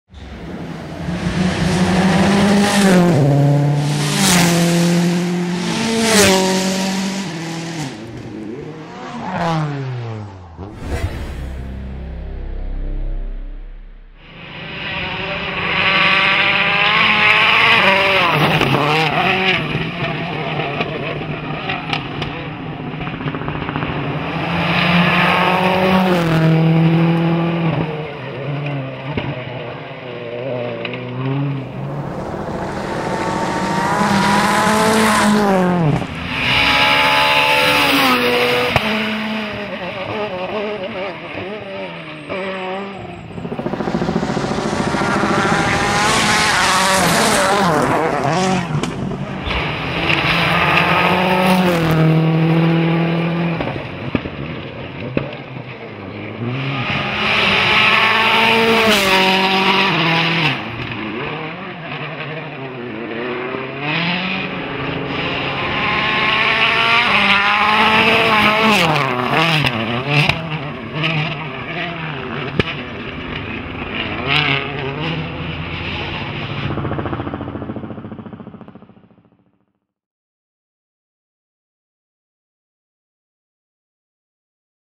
WRC GB 2010 - Day 3 Preview
Here is the last preview before my feature video. Todays stages were Resolven and Margam park